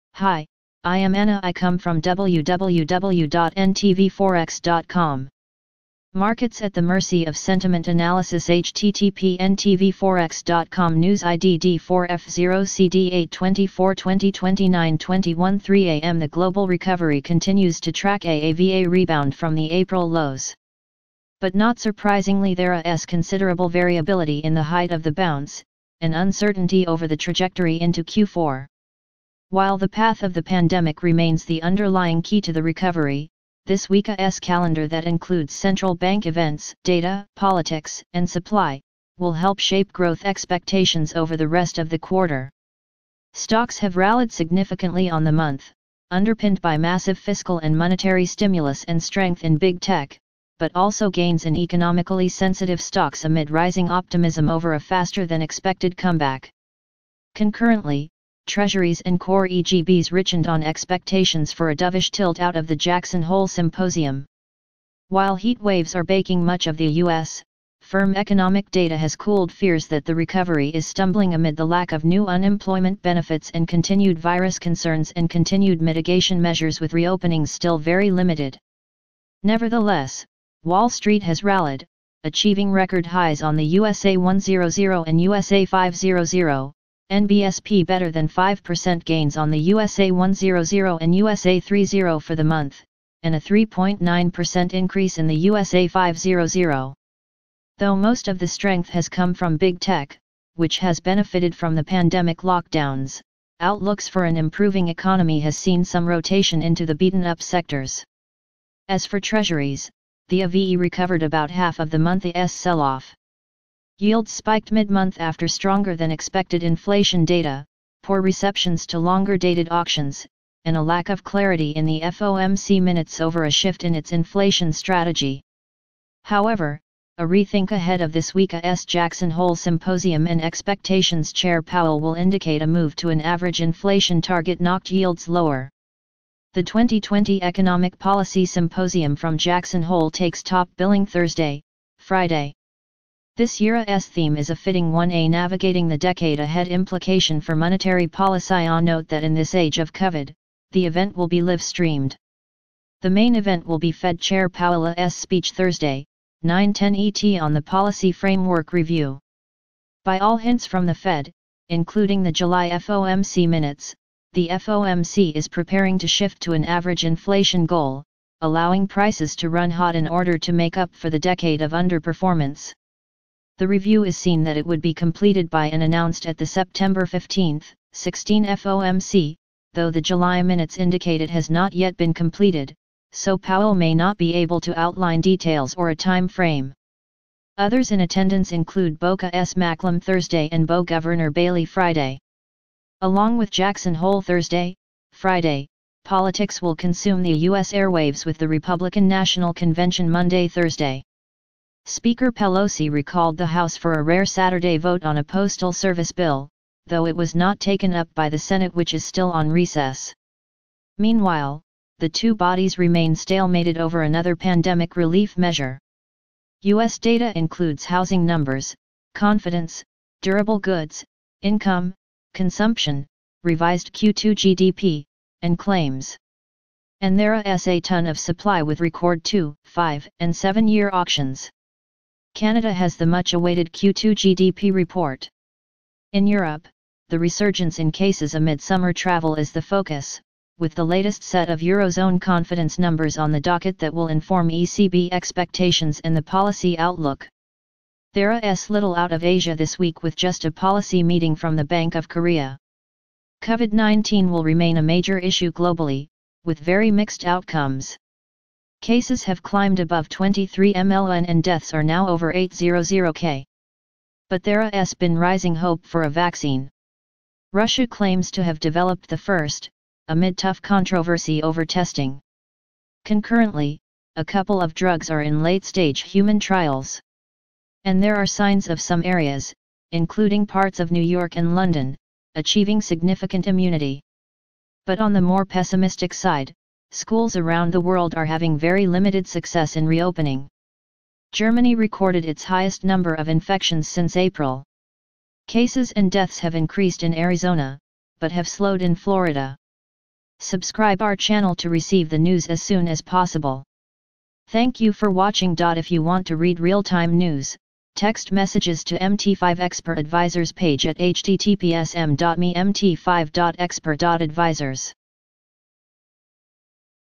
Analysis Markets at the mercy of sentiment
Markets at the mercy of sentiment Analysis
. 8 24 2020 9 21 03 AM The global recovery continues to track a V rebound from the April lows. But not surprisingly there s considerable variability in the height of the bounce, and uncertainty over the trajectory into Q4. While the path of the pandemic remains the underlying key to the recovery, this week s calendar that includes central bank events, data, politics, and supply, will help shape growth expectations over the rest of the quarter. Stocks have rallied significantly on the month, underpinned by massive fiscal and monetary stimulus and strength in big tech, but also gains in economically sensitive stocks amid rising optimism over a faster than expected comeback. Concurrently, Treasuries and core EGBs richened on expectations for a dovish tilt out of the Jackson Hole symposium. While heat waves are baking much of the US , firm economic data has cooled fears that the recovery is stumbling amid th...